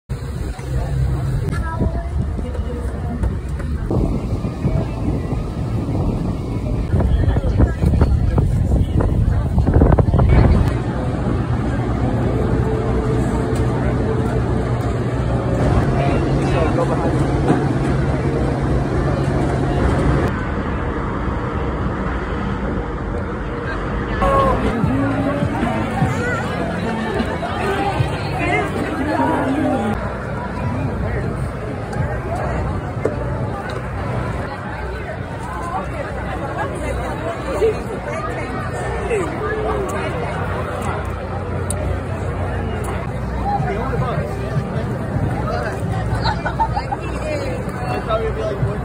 'It's surreal;' 5,000 sailors aboard USS George HW Bush return home
After more than an eight month deployment, 5,000 sailors aboard the USS George H.W. Bush returned home Sunday.

“It’s amazing, it’s a bittersweet moment, it’s surreal”, said sailor Sydney Williams, “You just never take these moments for granted.”